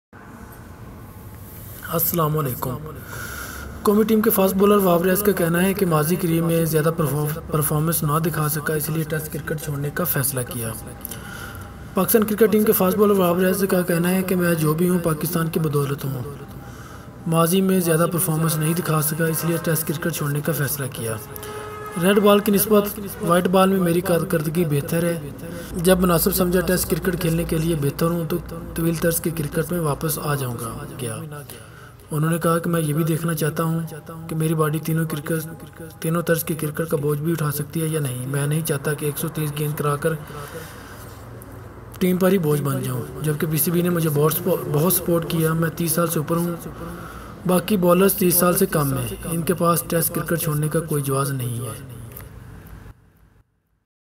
Wahab Riaz test cricket se retire ٹیسٹ کرکٹ چھوڑنے کا فیصلہ کیا، وہاب ریاض
Wahab Riaz test cricket se retire
کارکردگی نہ ہونے کے سبب ٹیسٹ کرکٹ چھوڑنے کا فیصلہ کیا، وہاب ریاض
studio 64